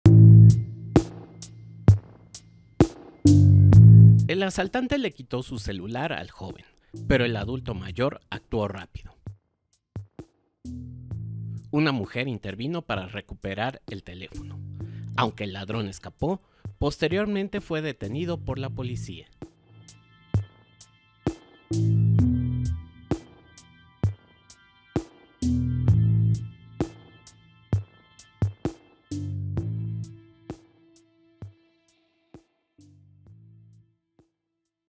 Hombre de 72 años derriba a asaltante que atracó a joven - N+
Llaman “héroe” a un hombre de 72 años que vio cómo asaltaban a un joven, y, en el momento preciso, derribó al ladrón. El agitado momento fue grabado en Rusia el 23 de octubre.
Con ayuda de una mujer, lograron recuperar el celular del joven. Sin embargo, el asaltante huyó. No obstante, la policía logró dar con el delincuente, quien ahora es investigado por las autoridades locales por el delito de robo con uso de violencia que no amenaza la vida.